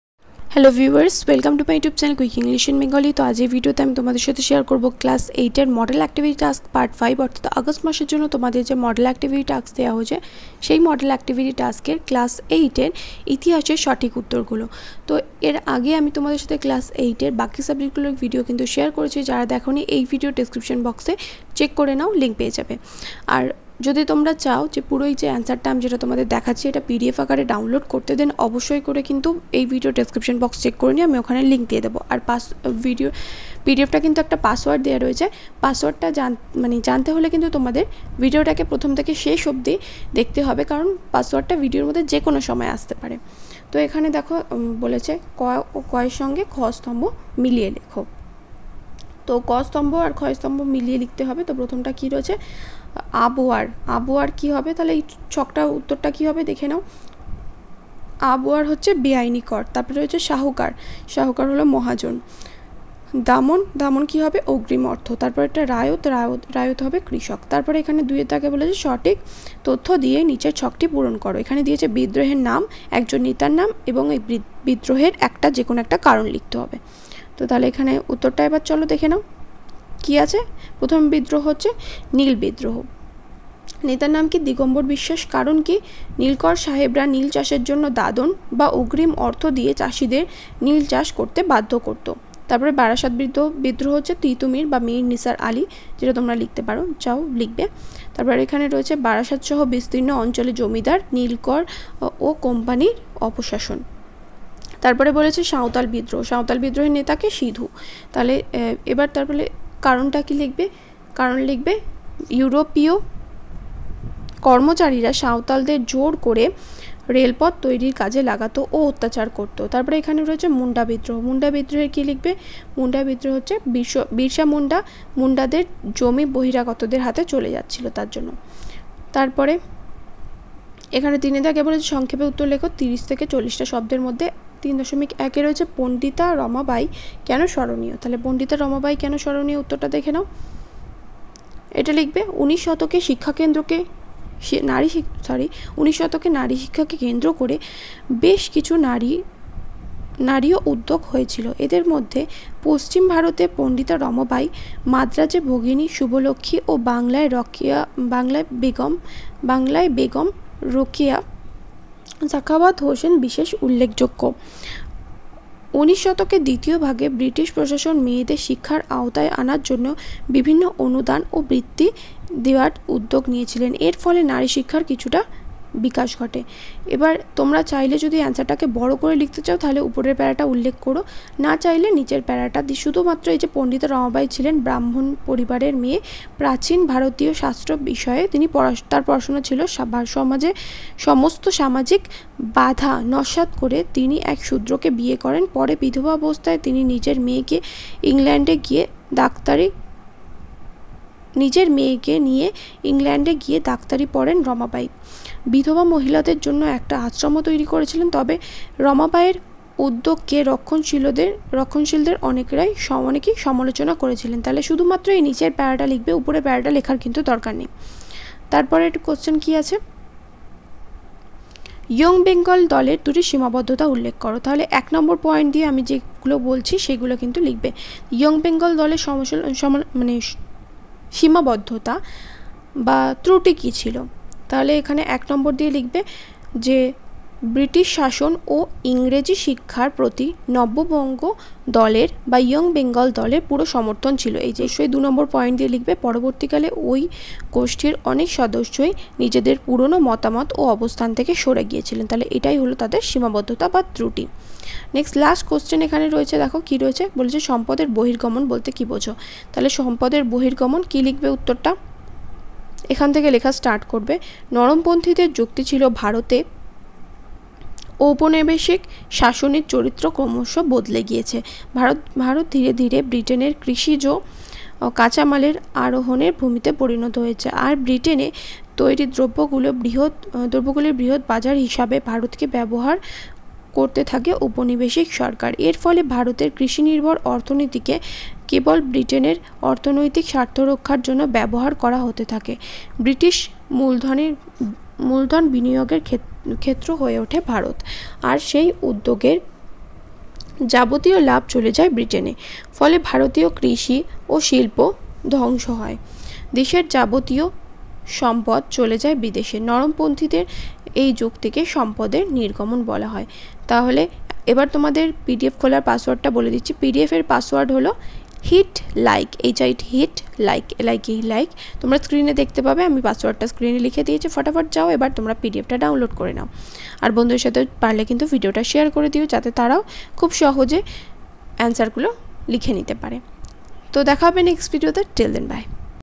Class 8 History Model Activity Task Part 5 For August |Second Section 2021
Hello Everyone, I am Rima and today in this video I am going to share new model activity task of class 8.